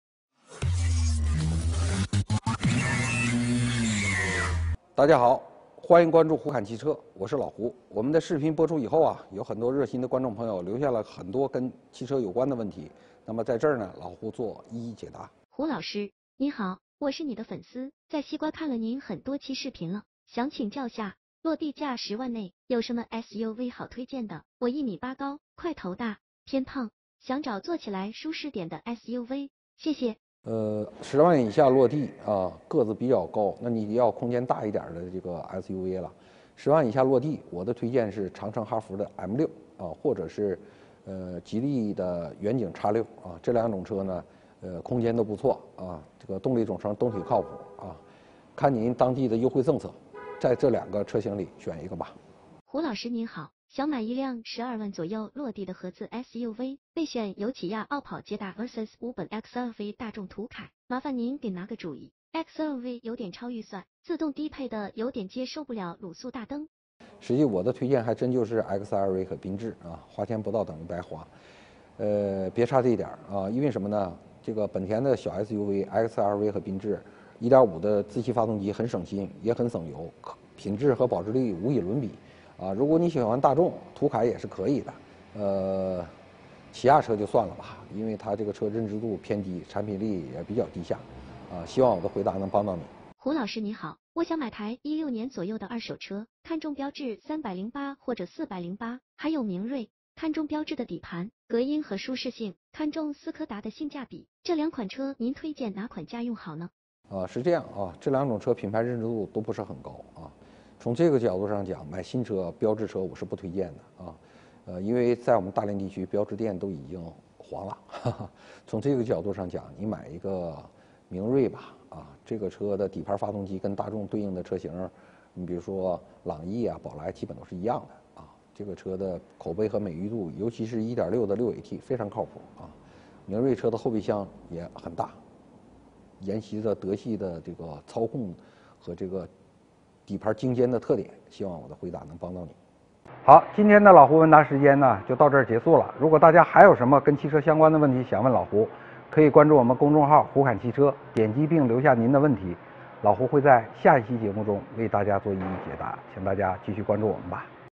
十万预算有什么SUV值得入手？老胡推荐这几款，空间都很大
车系： 哈弗M6 远景X6 本田XR-V 缤智 明锐